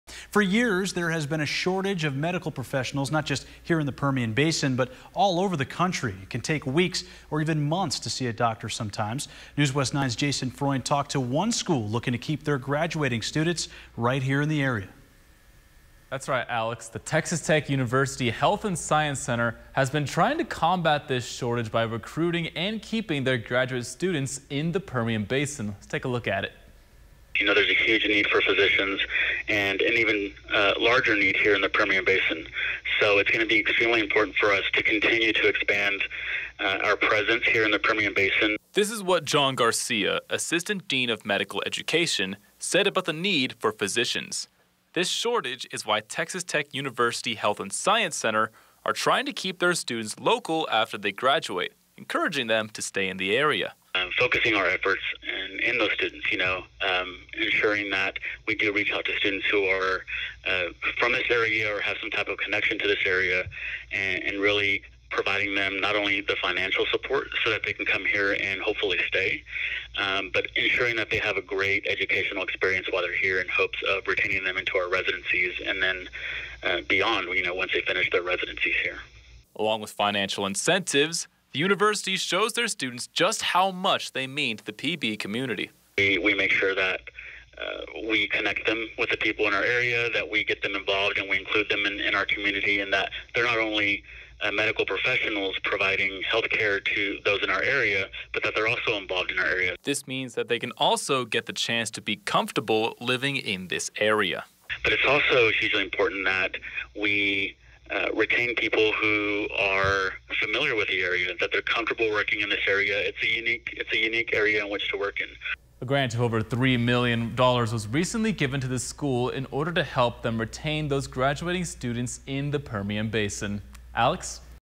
Texas Tech University Health Sciences Center looks to retain graduate medical students
Keeping graduate medical students will hopefully combat the growing lack of medical professionals in the Permian Basin.